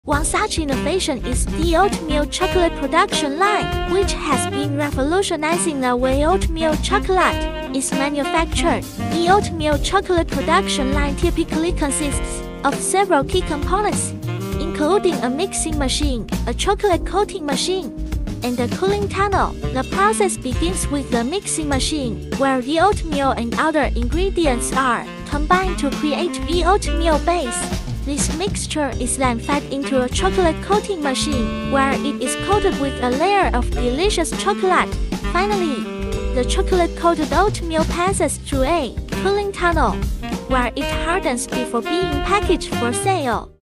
HTL T888 -Oat chocolate production line
🪆 The oatmeal chocolate production line typically consists of several key components, including a mixing machine, a chocolate coating machine, and a cooling tunnel.

    🪆 The process begins with the mixing machine, where the oatmeal and other ingredients are combined to create the oatmeal base.

🪆 This mixture is then fed into the chocolate coating machine, where it is coated with a layer of delicious chocolate.

   🪆 Finally, the chocolate-coated oatmeal passes through a cooling tunnel, where it hardens before being packaged for sale.